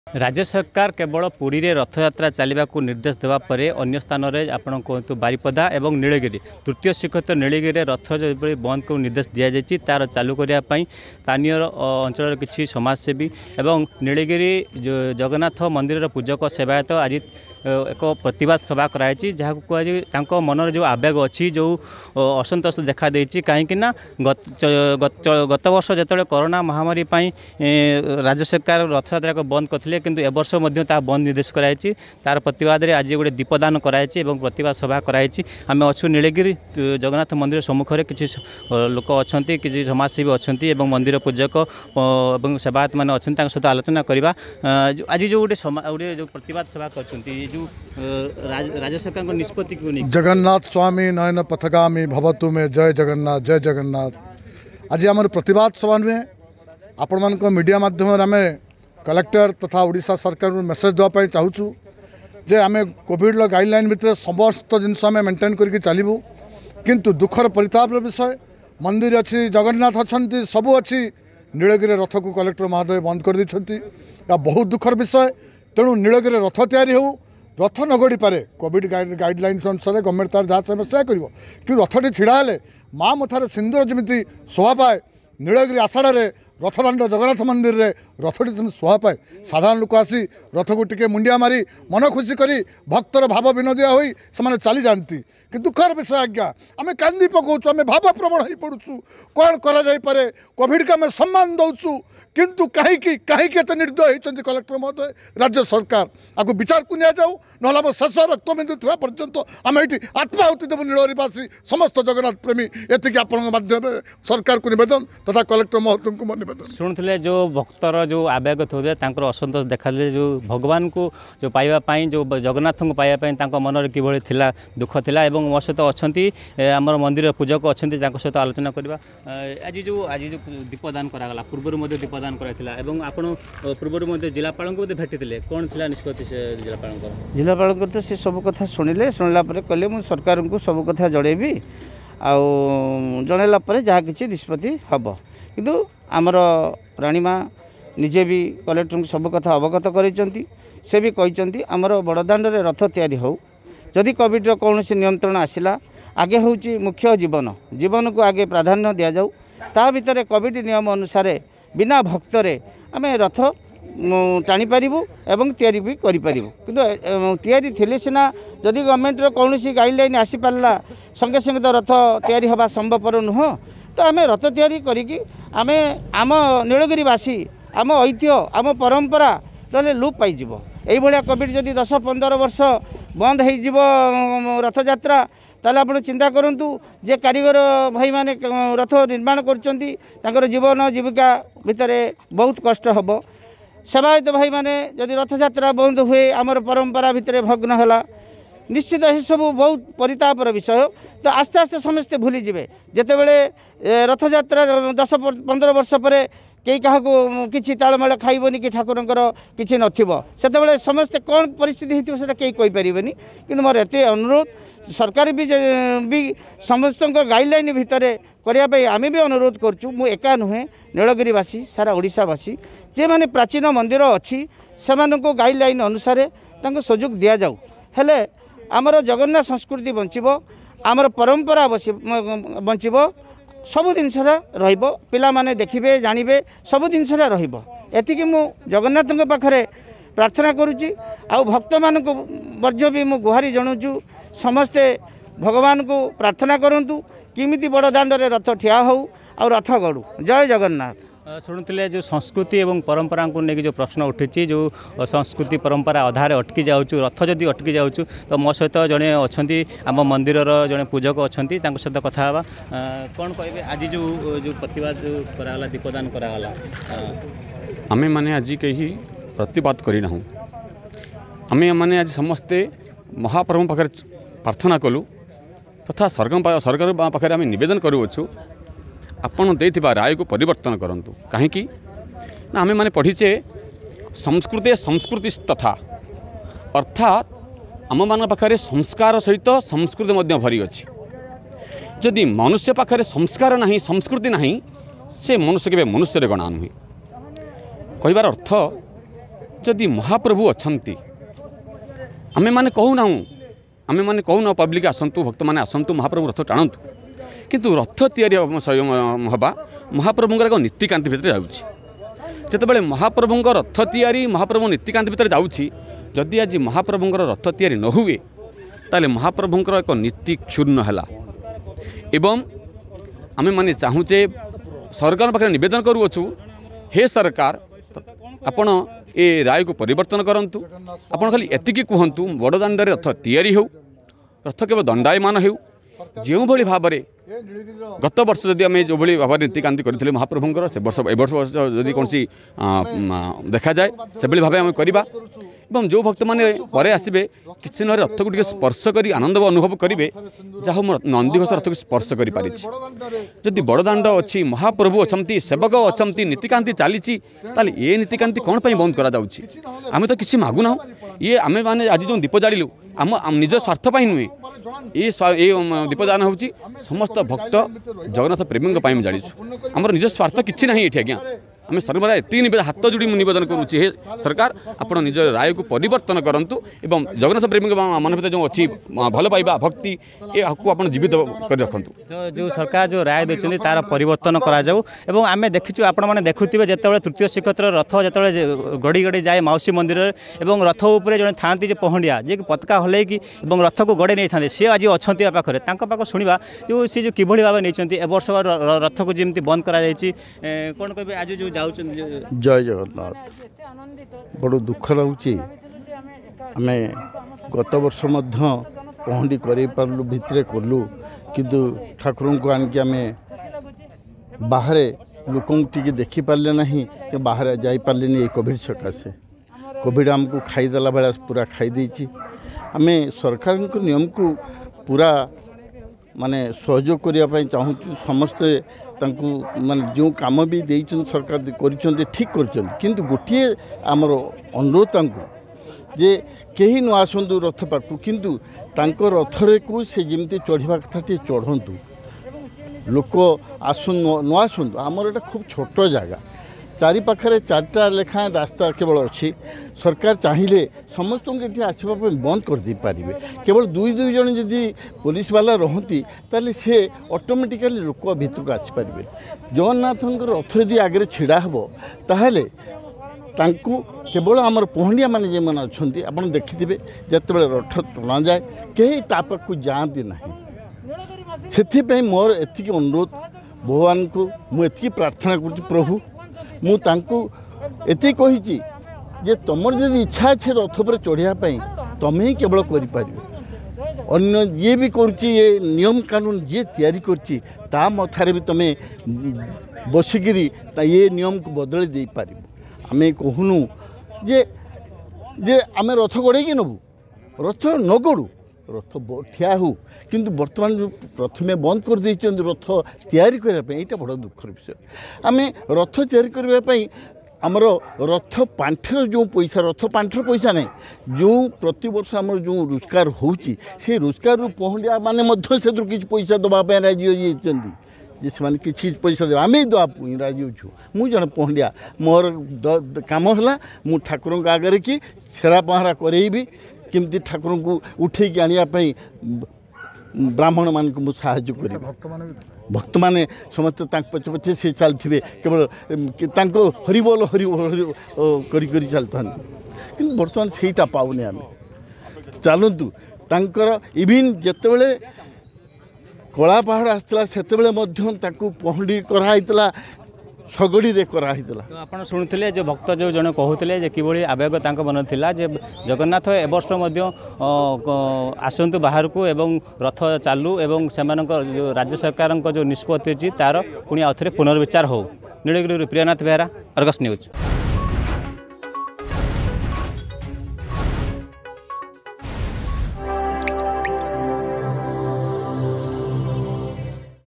ନୀଳଗିରିର ରଥଯାତ୍ରା ବନ୍ଦକୁ ନେଇ ପ୍ରତିବାଦ
ରାଜ୍ୟ ସରକାରଙ୍କ ରଥଯାତ୍ରା ବନ୍ଦ ନିର୍ଦ୍ଦେଶକୁ ନେଇ ନୀଳଗିରିର କିଛି ସମାଜ ସେବୀ ପୂଜକ ଏବଂ ସେବାୟତମାନେ ଜଗନ୍ନାଥ ମନ୍ଦିର ସମ୍ମୁଖରେ ଦୀପ ଦାନ କରି ପ୍ରତିବାଦ କରିଛନ୍ତି ।